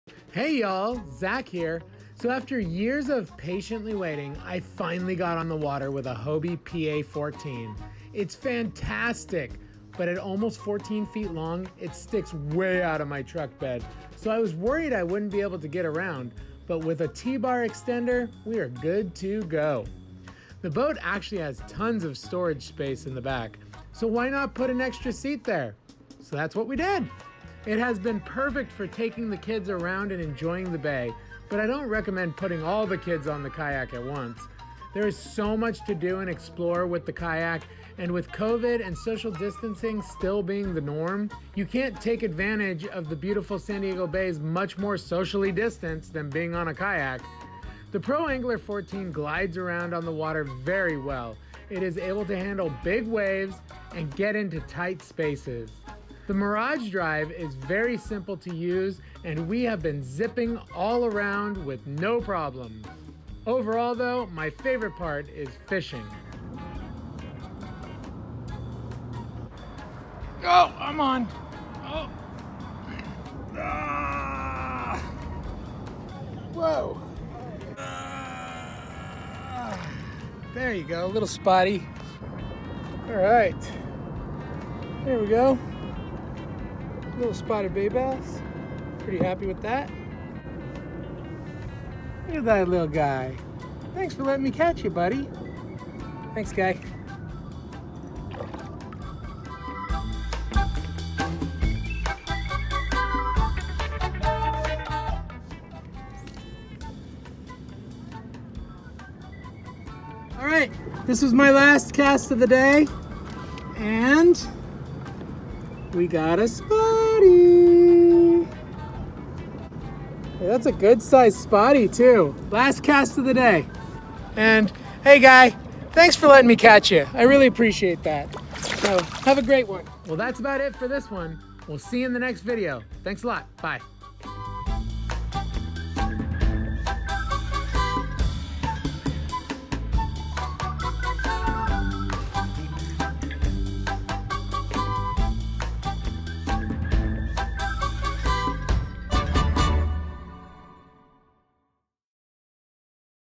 Fishing and Family Fun on our Hobie PA 14
We bought a Hobie Pro Angler 14 and we have been having a blast on the bays.